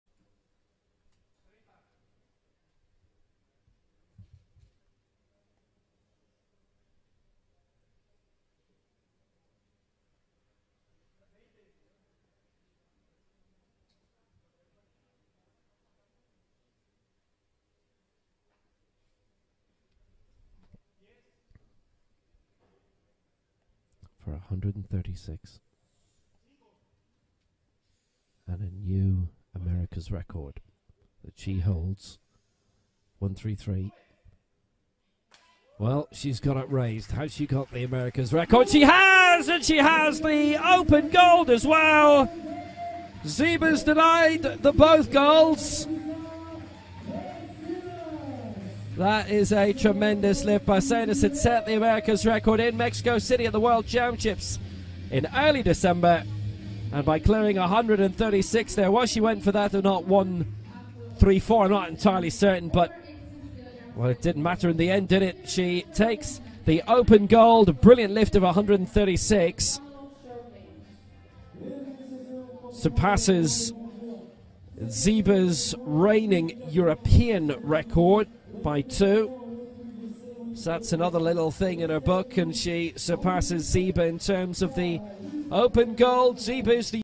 Perla Barcenas | Open Champion | Women's over 86kg | Berck-Sur-Mer 2018
Perla Barcenas from Mexico is the Open Champion in the women's over 86kg category with this lift of 136kg.

Silver: Marzena Zieba (Poland) 132kg
Bronze: Melaica Tuinfort (Netherlands) 118kg

Berck-Sur-Mer 2018 features as a mandatory step on the qualification pathway for all European Para powerlifters striving to qualify for the Tokyo 2020 Paralympic Games.

Like and follow World Para Powerlifting on Facebook and Instagram at: @ParaPowerlifting and follow on Twitter at: @Powerlifting